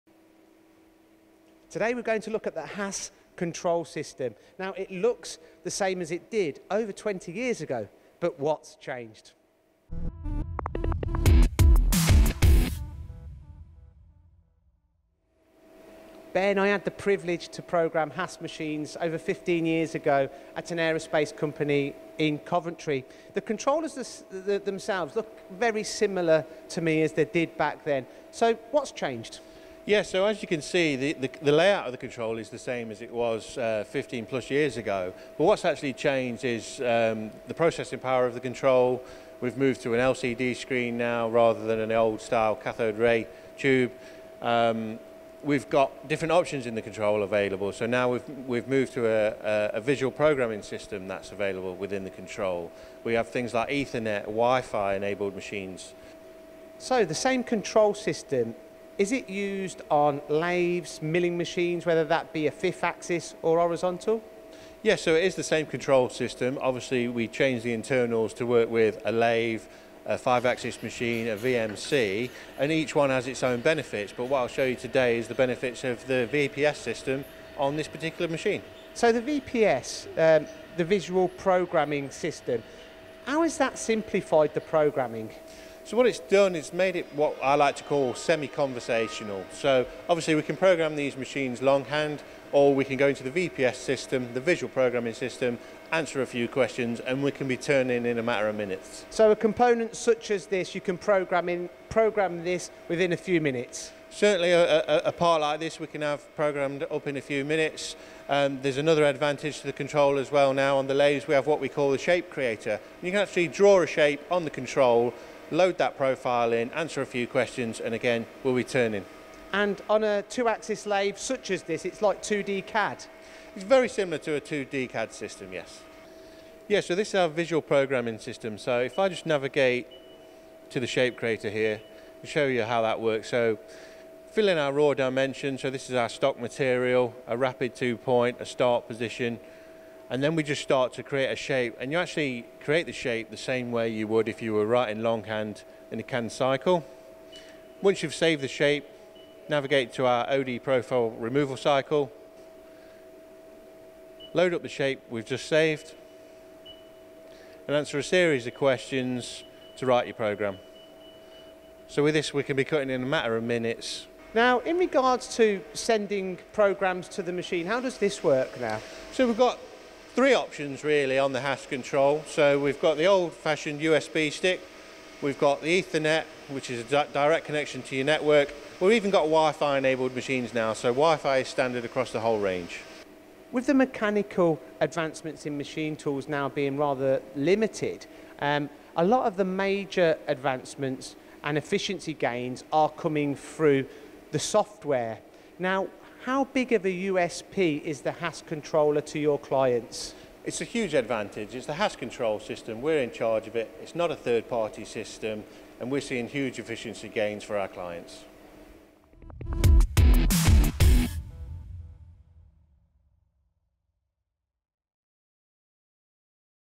Programming Made Easy With HAAS!
In a recent visit to the HAAS workshop in Leicester Gio discusses the evolution of the HAAS controller with Ben.
They discuss the software advancements which have simplified the way in which you can program directly into your machine tool and create programs within minutes.